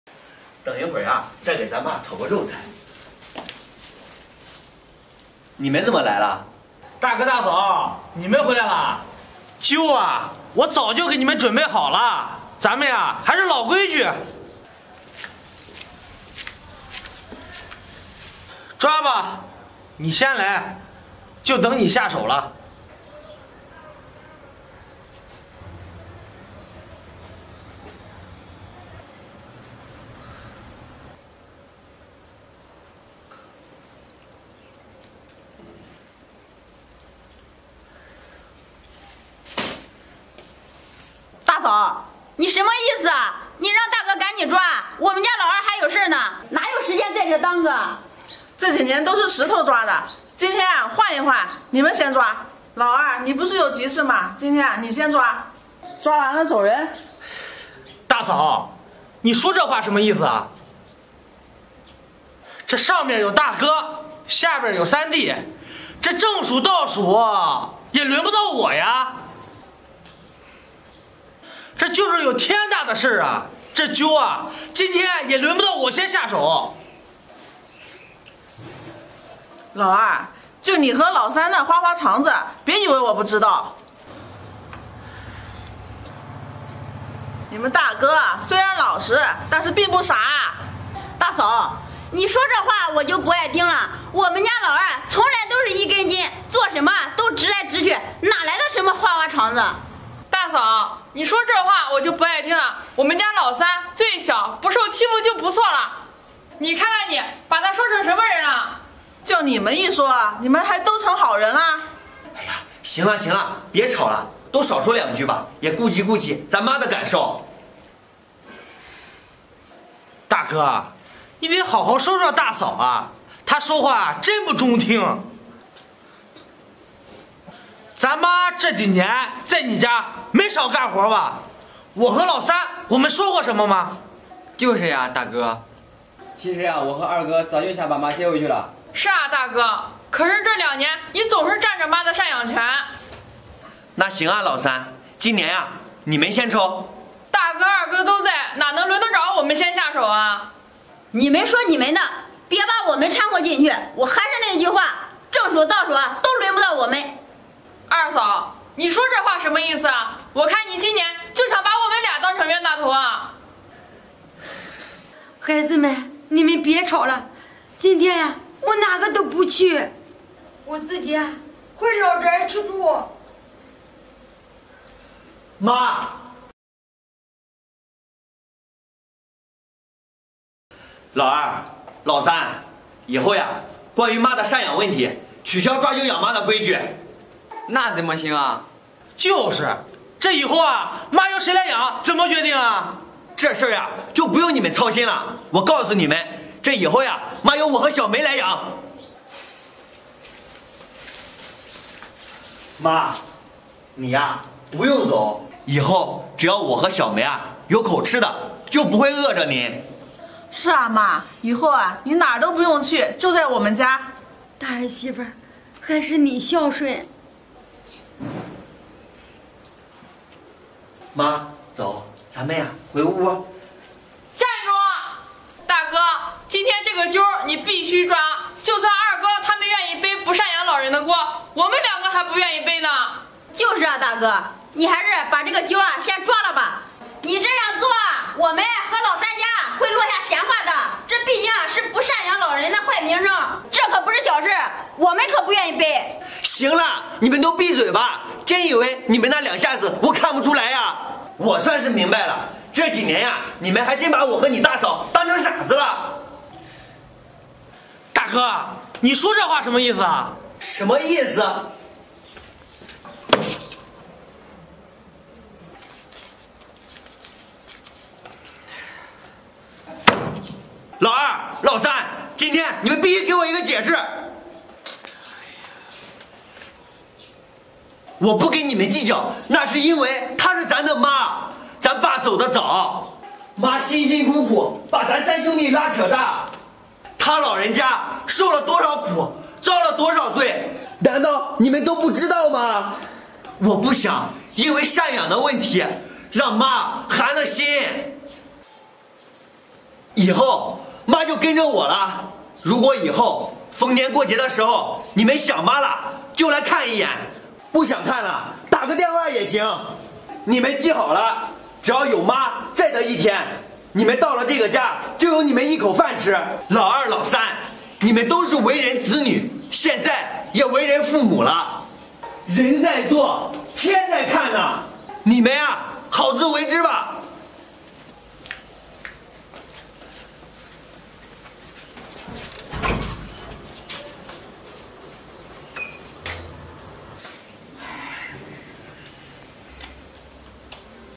兄弟俩欺负大哥老实，每年都在抓阄上动手脚，嫂子发飙了【阿东导演】
兄弟俩欺负大哥，嫂子实在看不下去了
短剧 兄弟 抓阄 我们的短剧是为了为观众提供娱乐，完全遵守平台的使用政策。